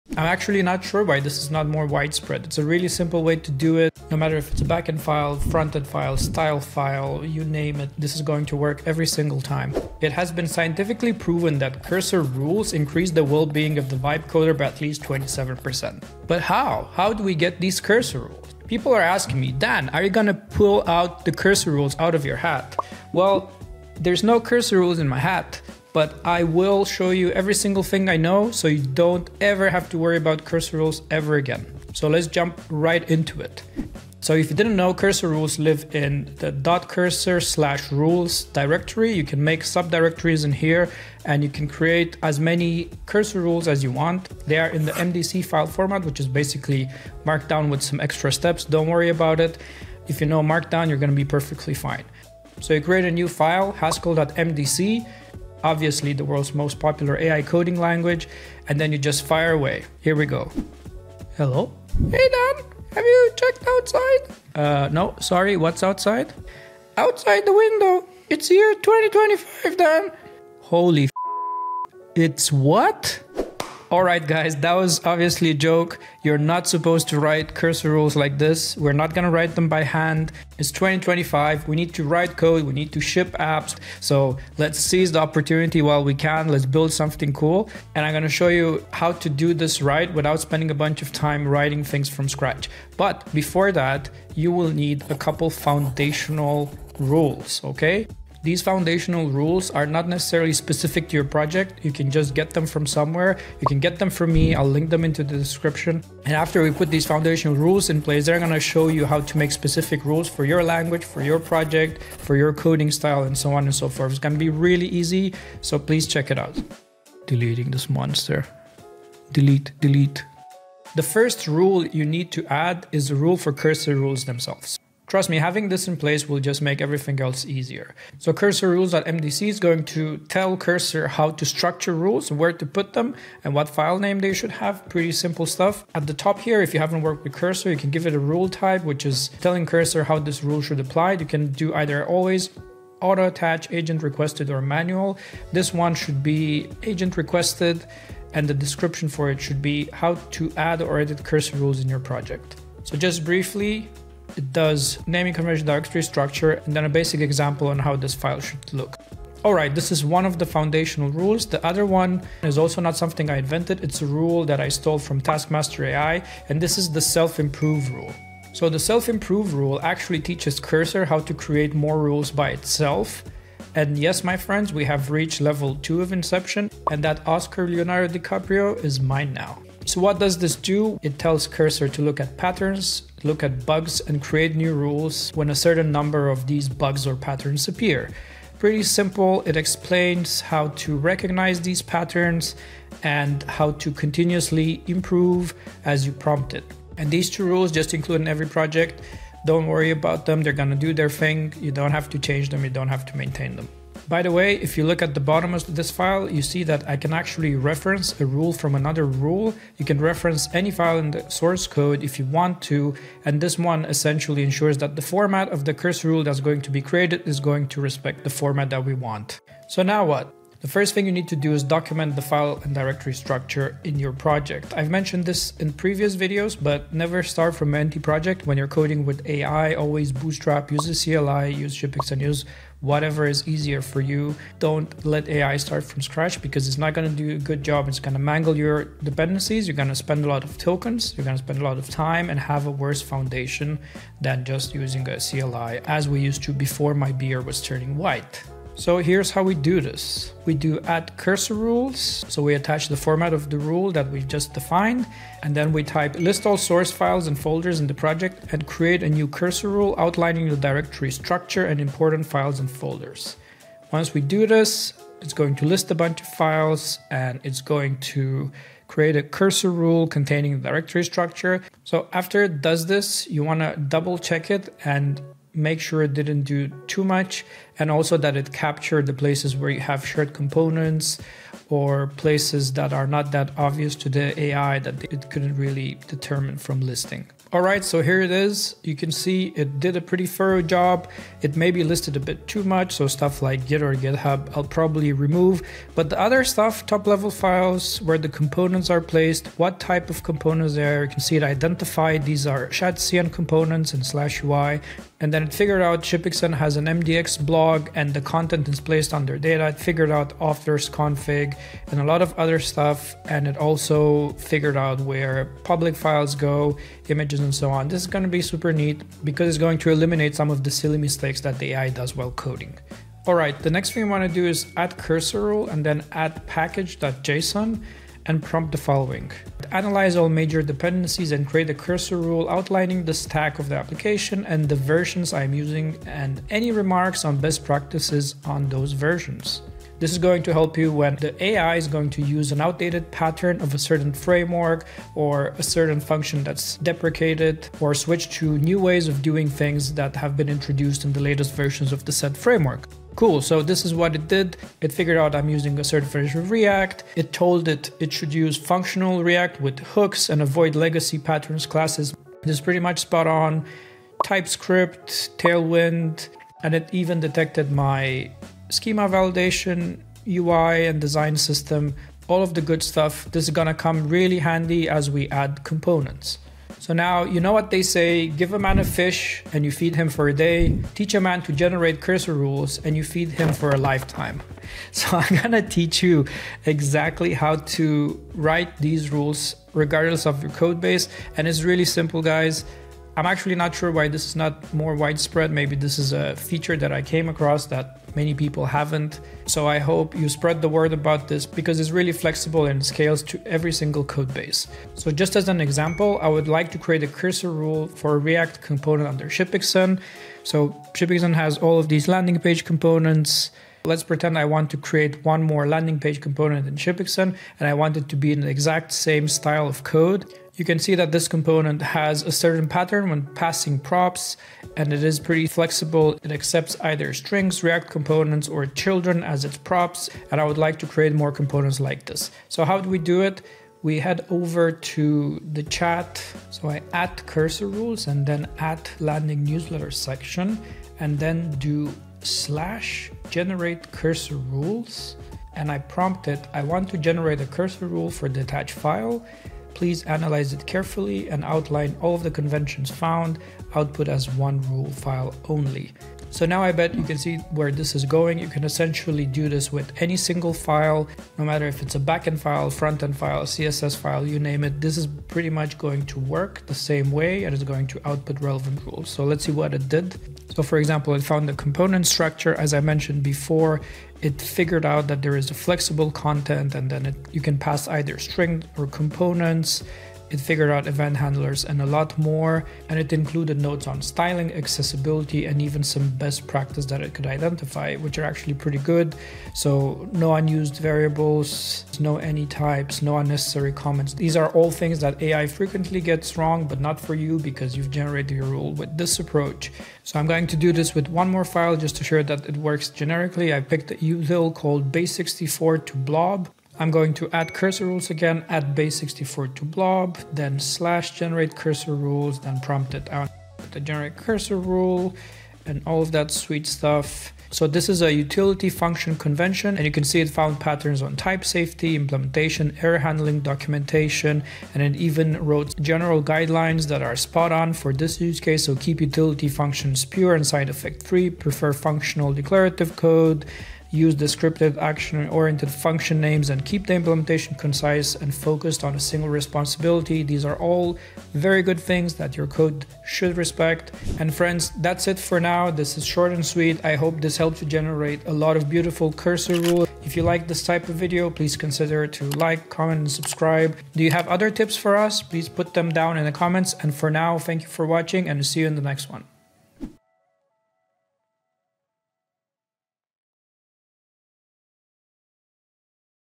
Cursor Rules guide for lazy devs. Setup rules in minutes with this quick tutorial!
Stop writing Cursor rules by hand / hunting them on Github. I share all prompts and teach you how to do this right 💪

Cursor rules help with silly mistakes and save a lot of time in the long run/
In this guide, I'll show you the best way to write them.

With this approach, you can automatically generate perfect Cursor rules for ANY project file, language, or framework.

✅ Set up foundational meta-rules that improve themselves
✅ Auto-document your project structure
✅ Generate tech stack rules from package.json
✅ Use the `/Generate Cursor Rules` command for any file type
✅ Scale this approach to large codebases

By the end of this video, you'll have a repeatable system for creating project-specific coding guidelines that actually match YOUR codebase conventions.

**Timestamps:**
0:00 Silly Intro
1:35 Foundational Cursor Rules
2:03 Cursor Rules... rule
2:48 Self Improve Cursor Rule
4:16 Directory Structure Cursor rule
5:25 Tech Stack Cursor rule
6:46 React Cursor rule
8:28 Util Cursor rule
9:05 Conclusion

And that's the guide! You now have a pro Cursor setup, with the best rules without having to write one line of config!

for making this show happen!